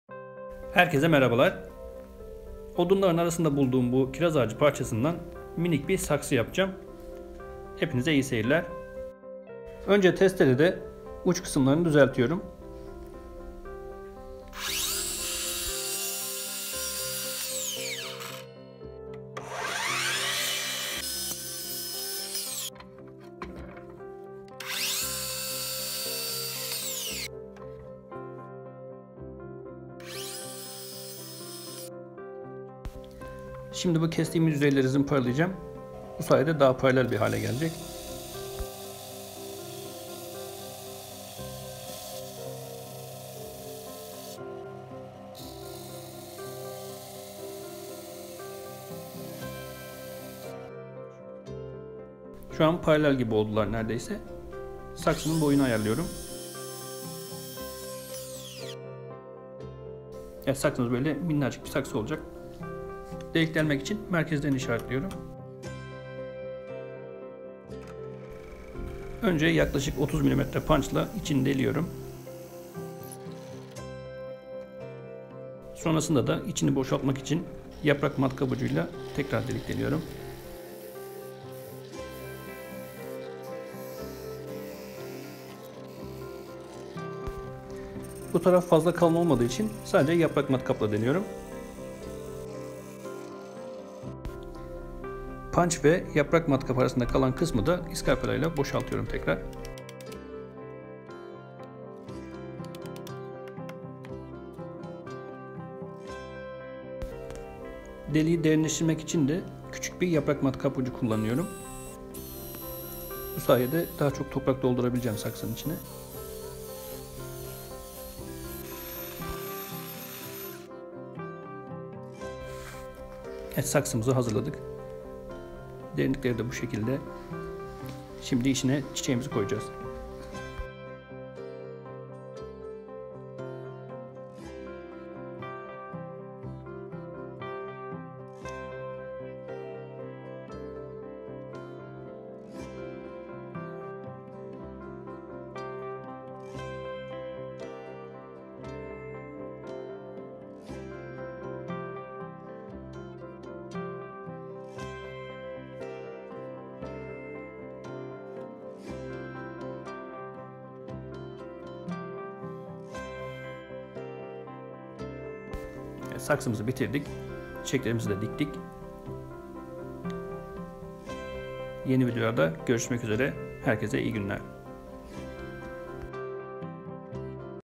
Skulent Saksısı Nasıl Yapılır?
Merhabalar,
Bu videoda odunların arasında bulduğum kiraz ağacını kullanarak, sukulent, kaktüs gibi küçük çiçekleri koyabileceğiniz bir saksının nasıl yapılabileceğini göstermeye çalıştım.